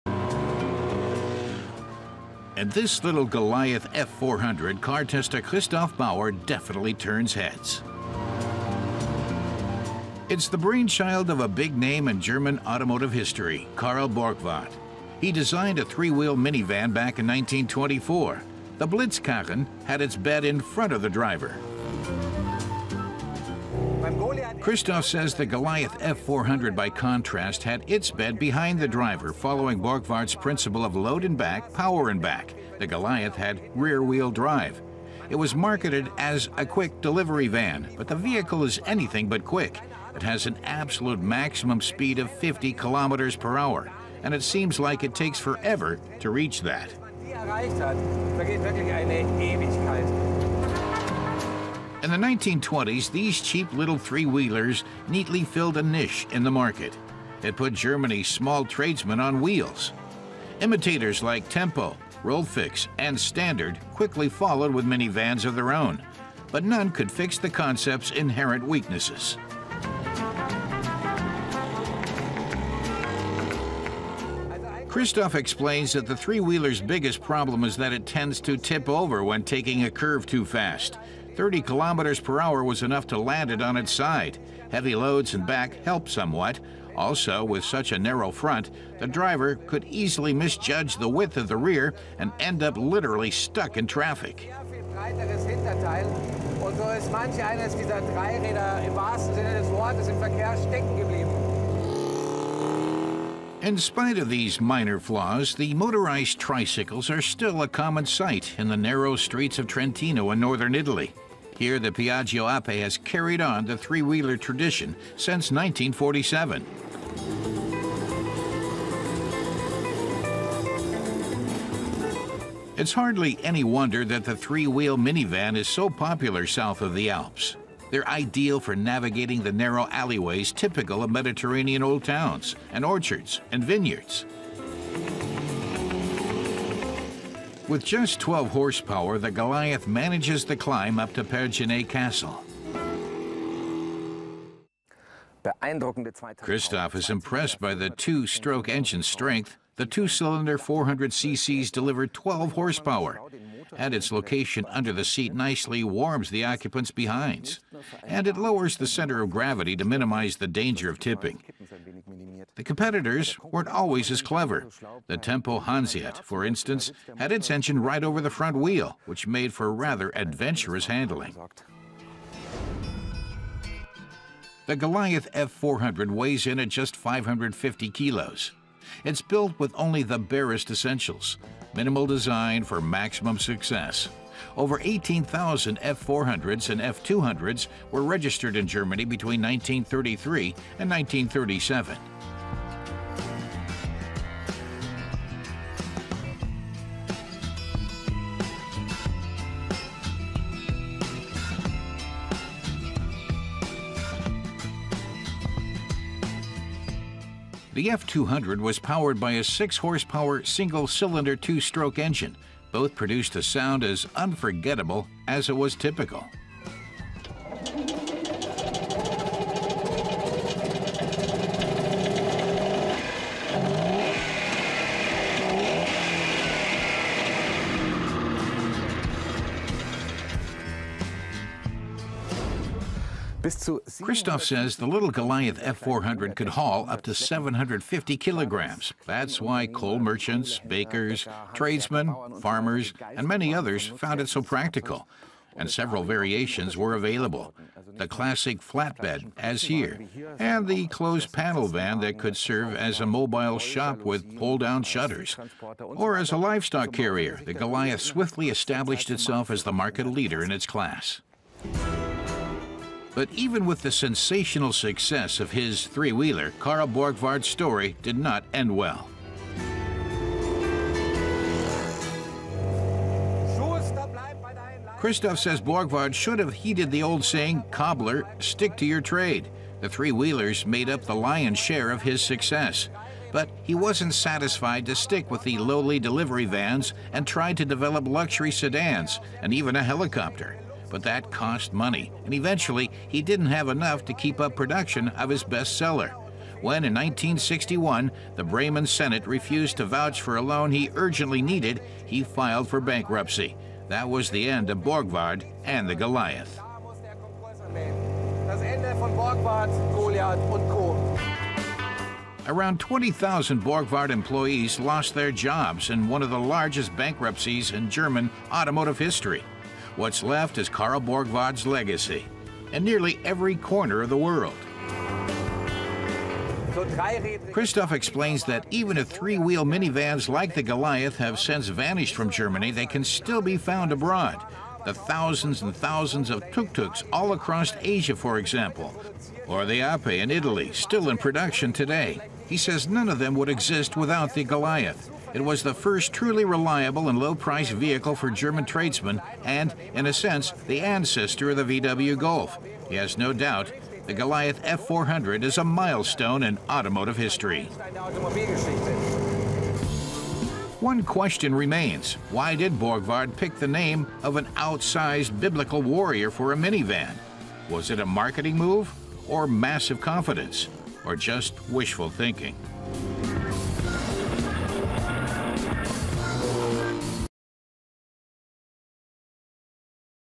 Three-wheeled history: Goliath F 400 | Vintage
It's a sensation testing the little Goliath F 400! This three-wheeler was developed by a key figure of the German automotive industry: Carl F. Borgward. In the 1920s, this low-priced three-wheeled vehicle filled a gap in the market, making Germany's small enterprise mobile!